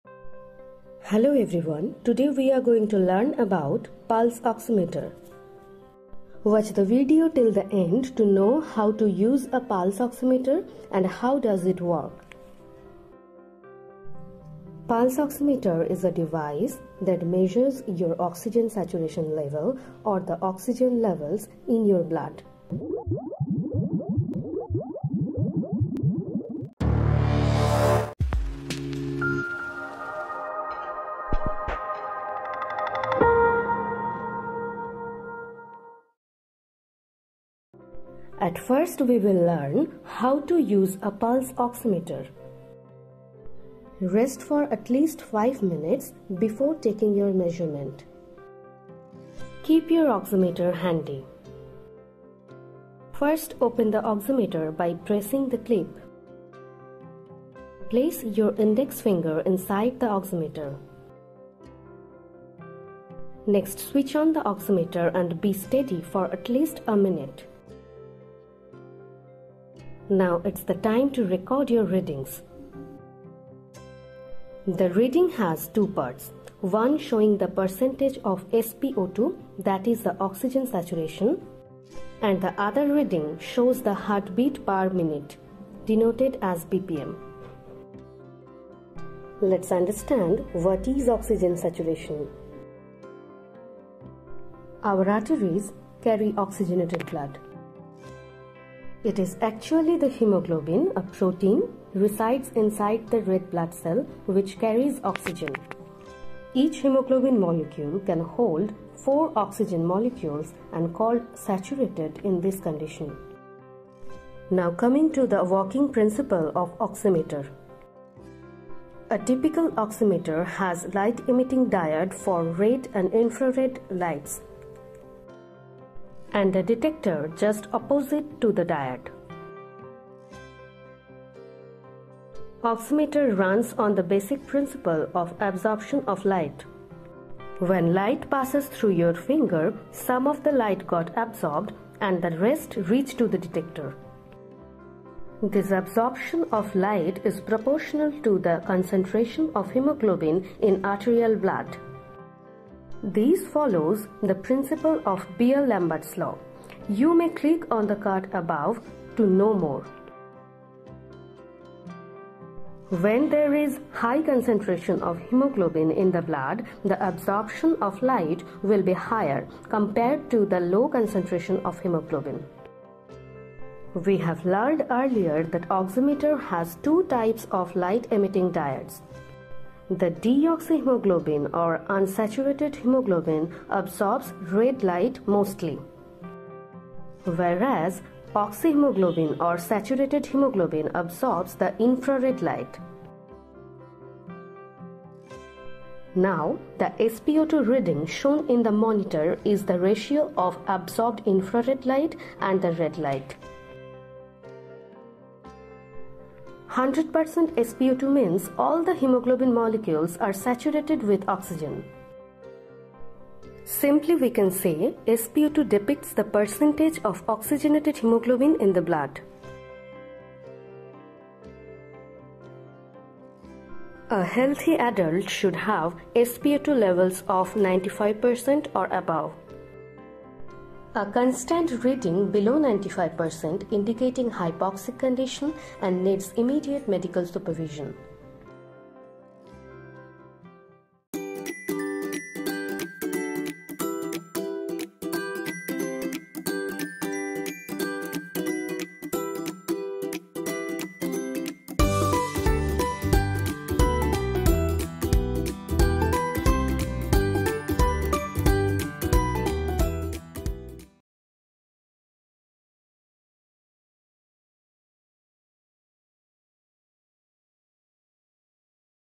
How Pulse Oximeter Works || How to use
biologyanimation Pulse oximetry is a noninvasive and painless test that measures your oxygen saturation level, or the oxygen levels in your blood. It can rapidly detect even small changes in how efficiently oxygen is being carried to the extremities furthest from the heart, including the legs and the arms.

The pulse oximeter is a small, clip-like device that attaches to a body part, like toes or an earlobe. It’s most commonly put on a finger, and it’s often used in a critical care setting like emergency rooms or hospitals. Some doctors, such as pulmonologists, may use it in office.